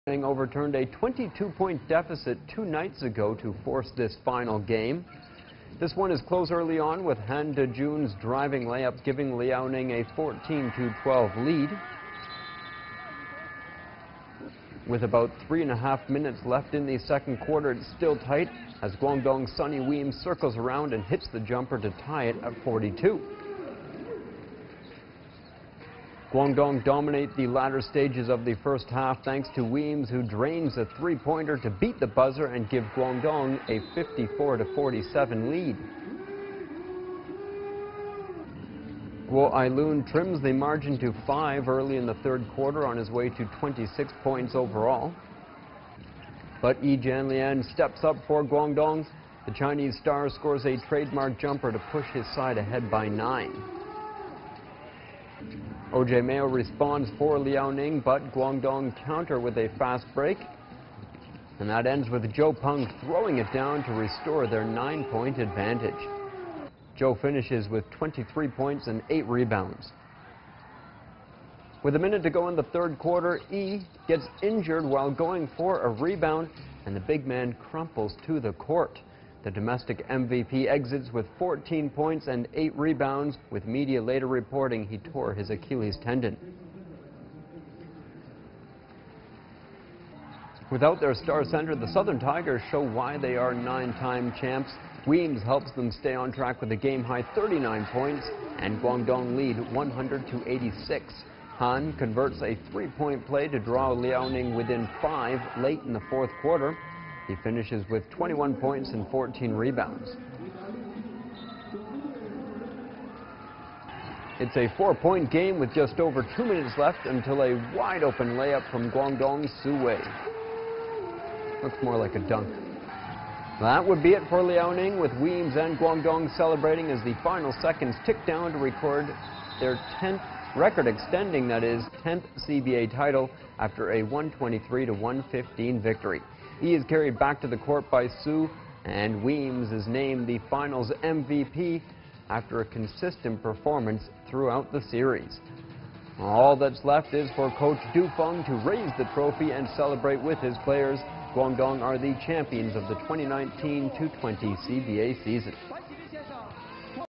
Guangdong Southern Tigers win 10th title with Weems named Finals MVP | CBA Finals
Defending champions the Guangdong Southern Tigers claimed their record extending tenth CBA title after a 123-115 victory. Sonny Weems is named the Finals MVP after a consistent performance throughout the series.